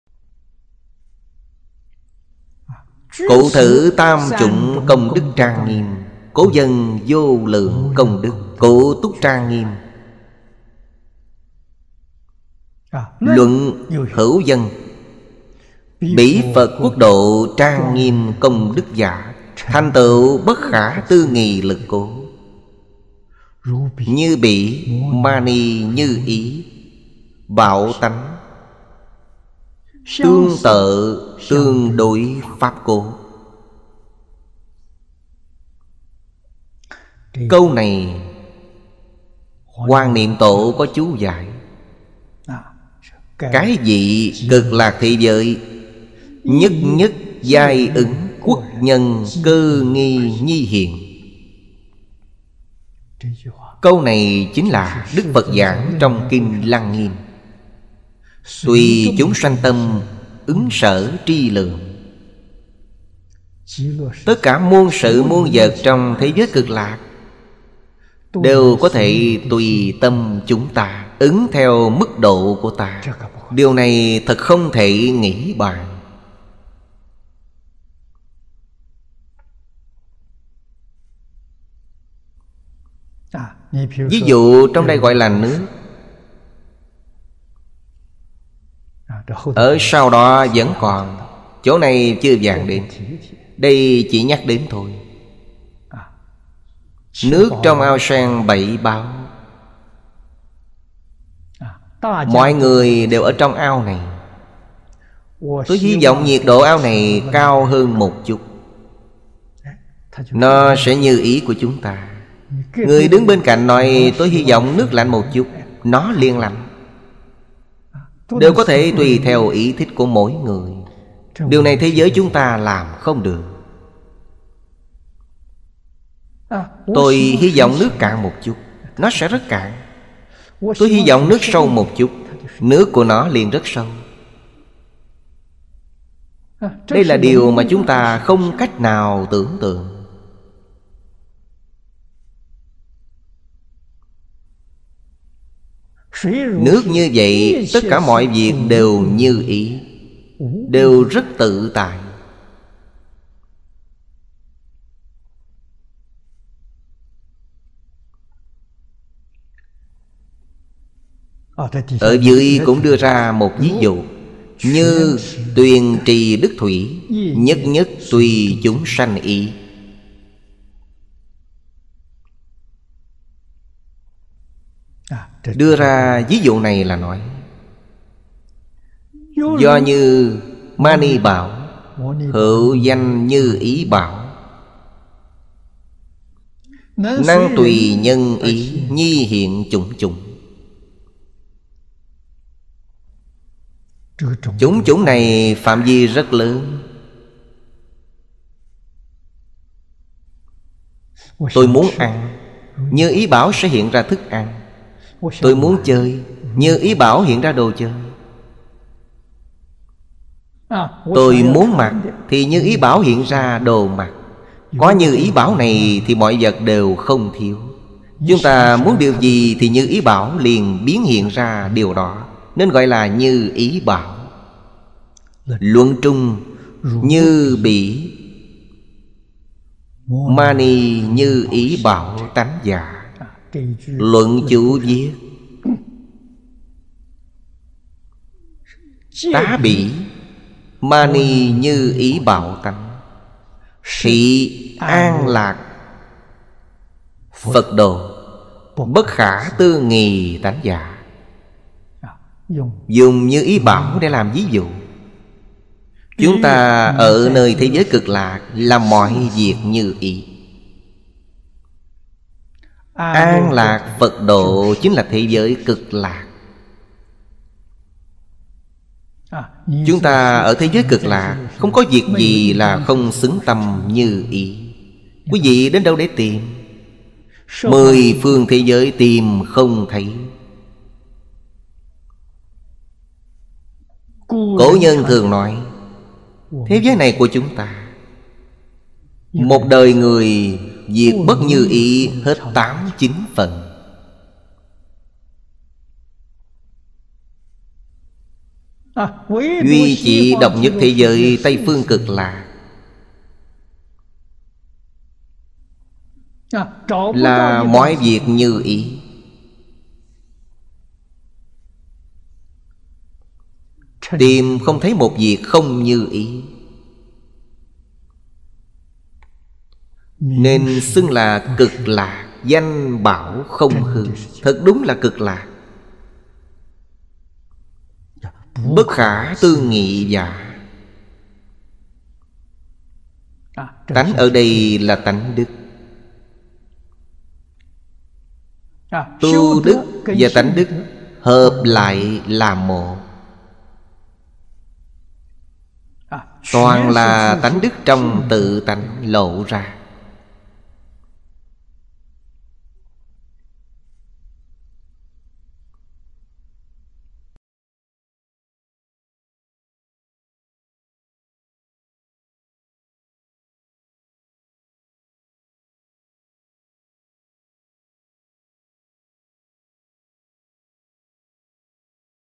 TĐ:2154-Tây Phương Cực Lạc Thế Giới Sự Sự Như Ý
Bài giảng:
“Cụ thử tam chủng công đức trang nghiêm. Cố vân, vô lượng công đức, cụ túc trang nghiêm. Luận hựu vân, bỉ Phật quốc độ trang nghiêm công đức giả, thành tựu bất khả tư nghì lực cố. Như bỉ ma ni như ý bảo tánh tương tợ tương đối pháp cố”. Câu này Hoàng Niệm Tổ có chú giải. “Cái vị Cực Lạc thế giới nhất nhất giai ứng quốc nhân cơ nghi nhi hiện”. Câu này chính là Đức Phật giảng trong kinh Lăng Nghiêm, “Tuỳ chúng sanh tâm, ứng sở tri lượng”. Tất cả muôn sự muôn vật trong thế giới Cực Lạc đều có thể tuỳ tâm chúng ta, ứng theo mức độ của ta. Điều này thật không thể nghĩ bàn.
      Ví dụ trong đây gọi là nước, ở sau đó vẫn còn, chỗ này chưa giảng đến. Đây chỉ nhắc đến thôi. Nước trong ao sen bảy báu, mọi người đều ở trong ao này. Tôi hy vọng nhiệt độ ao này cao hơn một chút, nó sẽ như ý của chúng ta. Người đứng bên cạnh nói  tôi hy vọng nước lạnh một chút, nó liền lạnh. Đều có thể tuỳ theo ý thích của mỗi người. Điều này thế giới chúng ta làm không được. Tôi hy vọng nước cạn một chút, nó sẽ rất cạn. Tôi hy vọng nước sâu một chút, nước của nó liền rất sâu. Đây là điều mà chúng ta không cách nào tưởng tượng. Nước như vậy, tất cả mọi việc đều như ý, đều rất tự tại.
      Ở dưới cũng đưa ra một ví dụ: “Như tuyền trì đức thuỷ, nhất nhất tuỳ chúng sanh ý”. Đưa ra ví dụ này là nói “Do như ma ni bảo, hựu danh như ý bảo, năng tuỳ nhân ý nhi hiện chủng chủng”. Chủng chủng này phạm vi rất lớn. Tôi muốn ăn, như ý bảo sẽ hiện ra thức ăn. Tôi muốn chơi, như ý bảo hiện ra đồ chơi. Tôi muốn mặc thì như ý bảo hiện ra đồ mặc. Có như ý bảo này thì mọi vật đều không thiếu. Chúng ta muốn điều gì thì như ý bảo liền biến hiện ra điều đó, nên gọi là Như ý bảo. “Luận trung, như bỉ ma ni như ý bảo tánh giả. Luận Chú viết, tá bỉ ma ni như ý bảo tánh, thị an lạc Phật độ bất khả tư nghì tánh dã”. Dùng như ý bảo để làm ví dụ. chúng ta ở nơi thế giới Cực Lạc là mọi việc như ý. An lạc Phật độ chính là thế giới Cực Lạc.
Đọc thêm ...